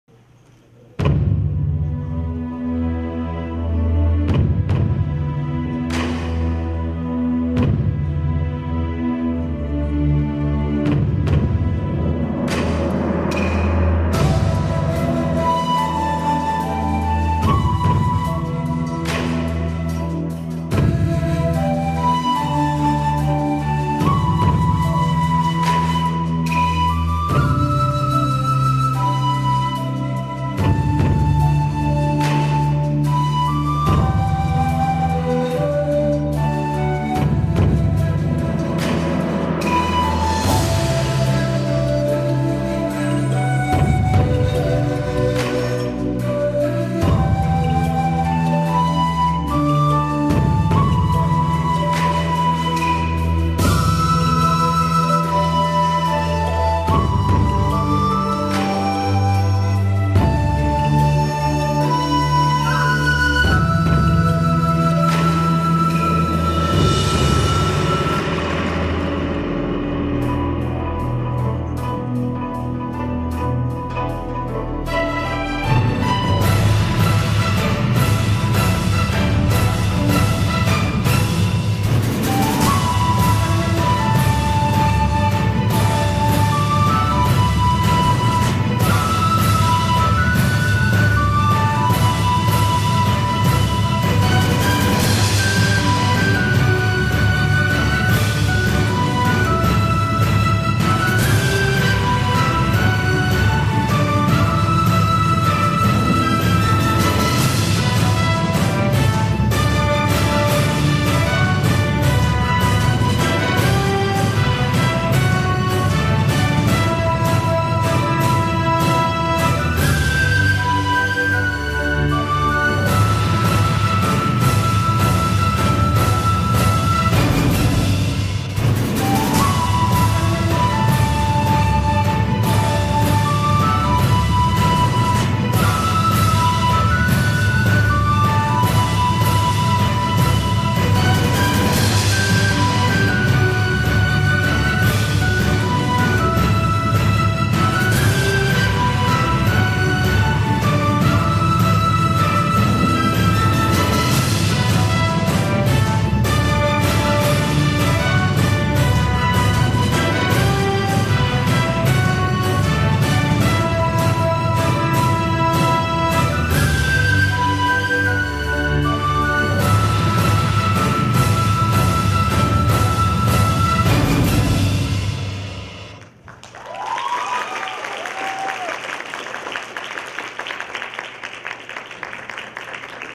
摩娑師生聯展｜2018冬｜中東肚皮舞：大唐天下
摩娑舞團：
中東肚皮舞、部落融合風、寶萊塢印度舞、瑜伽、肢體開發

摩娑舞團2018冬季師生聯展：舞神攻略
時地：2018年12月30日於民生社區活動中心
指導老師：Cathy老師
表演舞者：東湖假日班